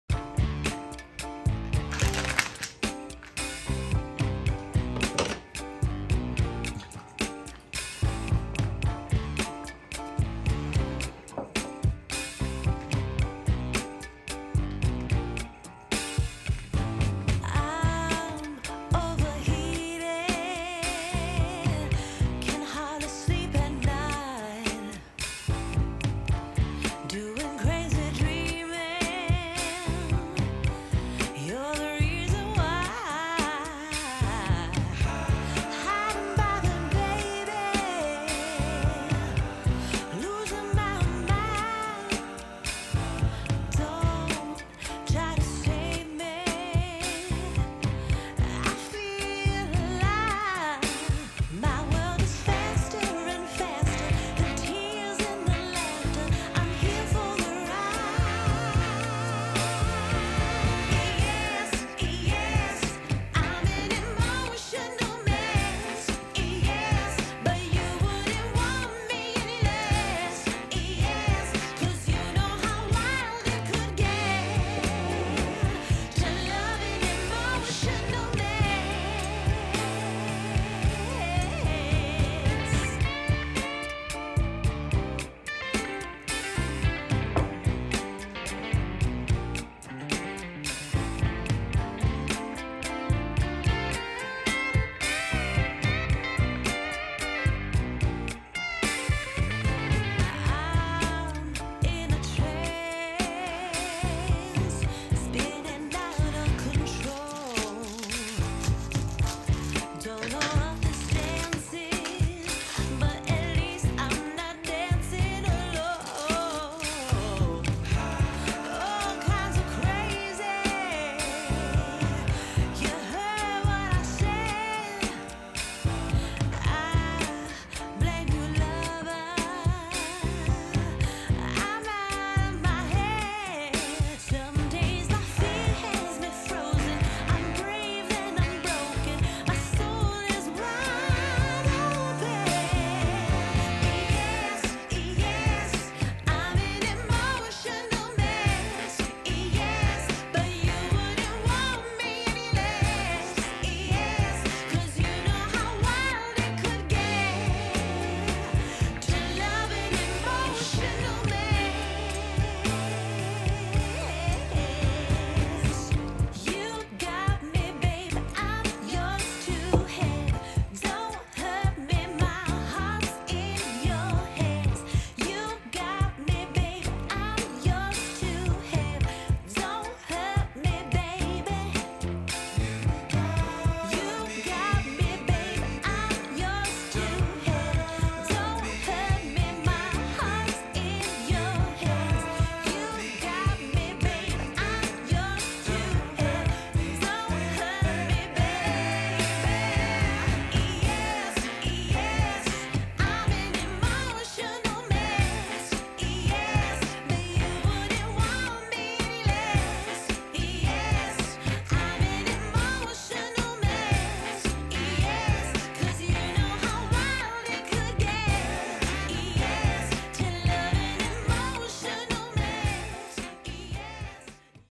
ПРАЗДНИЧНАЯ НАРЕЗКА колбасы и сыра, которая УДИВИТ ГОСТЕЙ
Красивая нарезка на праздничный стол, которая точно удивит гостей! Сырная тарелка, мясная тарелка – такие розы подойдут и для их украшения.

И не только на новогодний стол, кстати, но и на любой другой праздник (например, на 8 марта)

Что нужно:

салями, сальчичон или сервелат (любая тонкая колбаса подойдет, которая не рвется при сворачивании)
плавленный сыр в пластинах
салатная зелень (базилик, салат романо и т д)
шпажки
сыр моцарелла